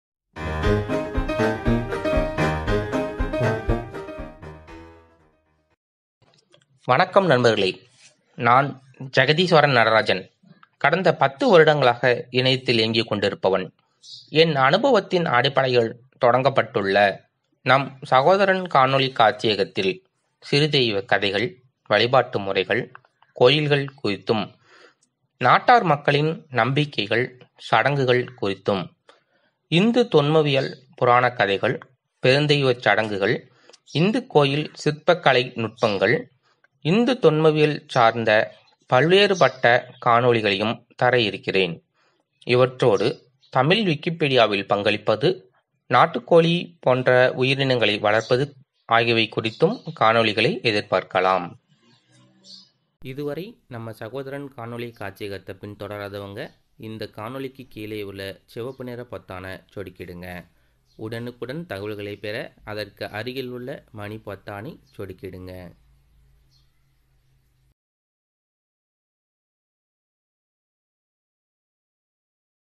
சகோதரன் காணொளிக் காட்சியகம் - அறிமுகம் Intro of Sagotharan Kaanoli Kaachiyagam
சகோதரன் காணொளிக் காட்சியகம் அறிமுகம்

வணக்கம் நண்பர்களே, நான் ஜெகதீஸ்வரன் நடராஜன். கடந்த பத்து வருடங்களாக இணையத்தில் இயங்கிவருகிறேன். கீற்று, வார்ப்பு போன்ற இணைய இதழ்களில் என் படைப்புகள் இடம் பெற்றுள்ளன. தமிழ் விக்கிப்பீடியாவில் கட்டுரையாக்கம், திருத்தம், புகைப்பட சேர்க்கை ஆகியவற்றை செய்து தற்போது நிர்வாகி பொறுப்பில்  உள்ளேன்.

என் அனுபவத்தின் அடிப்படையில் நான் கற்று அறிந்த தகவல்களை காணொளிகளாக இங்கு தர இருக்கிறேன். வருங்கால சந்ததியினருக்கு தரமான உண்மையான தகவல்களை இந்த காட்சியகம் வழங்கும் என நம்புகிறேன். நன்றி.